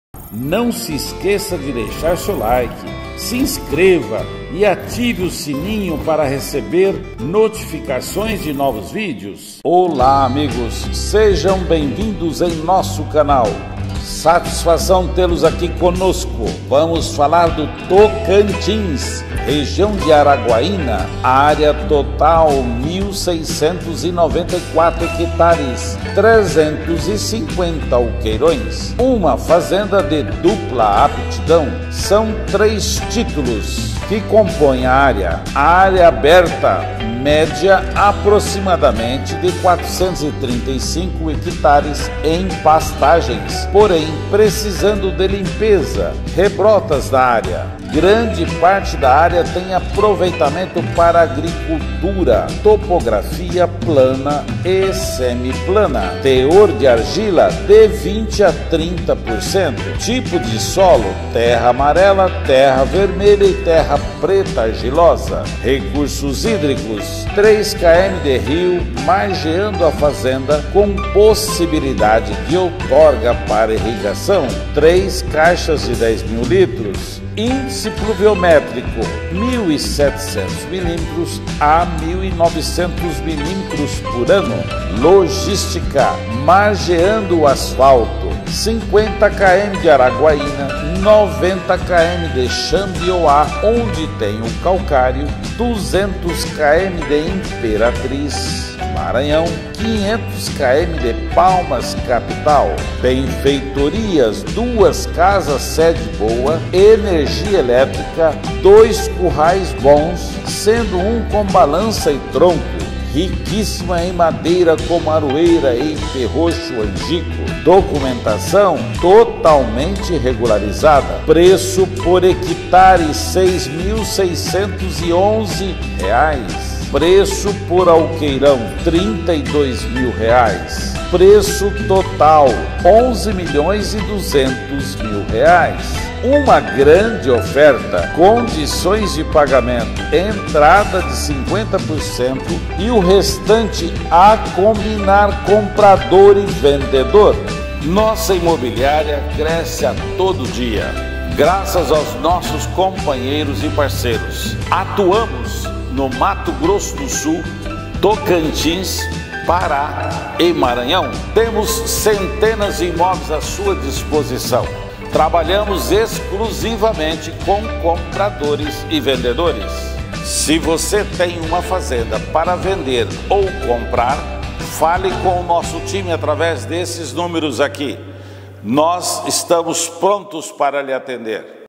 SUPER OFERTA em ARAGUAÍNA-TO 1.694 Hec FAZENDA p/ CRIA, RECRIA E ENGORDA OU p/ AGRICULTURA
Localização: REGIÃO DE ARAGUAINA, TOCANTINS
Área total: 1.694 Hectares (350 Alqueirões)
Dupla Aptidão
São 03 Títulos sendo 01 (435,6 Hectares), 01 (726 Hectares) e 01 (532,4 Hectares)
Área Aberta: Média de 435,6 Hectares em pastagens, porém precisando de limpeza.
Grande parte da área tem aproveitamento p/ agricultura/lavoura.
Topografia: Plana
Teor de Argila: 20% à 30%
Tipo de Solo: Terra amarela, terra vermelha e terra preta argilosa.
Recursos Hídricos:
3 Km de rio margeando a fazenda com possibilidade de outorga para irrigação
3 caixa de 10 mil litros.
Índice pluviométrico: 1.700 mm à 1.900 mm\ano

Logística:
Margeando o asfalto
50 Km de Araguaína-TO
90 Km Xambioa (calcário)
200 Km de Imperatriz-MA
500 Km de Palmas-TO (capital)

Benfeitorias:
02 Casas sedes boa, Energia elétrica,
2 currais bom, sendo um c/ balança e tronco.
Rica em madeira como aroeira e ipê roxo, angico etc...

Documentação: 100% Regularizada
Preço por hectare: R$ 6.611,00
Preço po Alqueirão: R$ 32.000,00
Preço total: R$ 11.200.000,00
Condições de pagamento: Entrada de 50%, restante em até 1 ano.

⚜Anuncie sua fazenda conosco, estamos em todas as plataformas digitais, Youtube, Instagram, Whatsapp, Facebook, LinkedIn, Olx, Mercado Livre,  Zap, Viva real e Site.⚜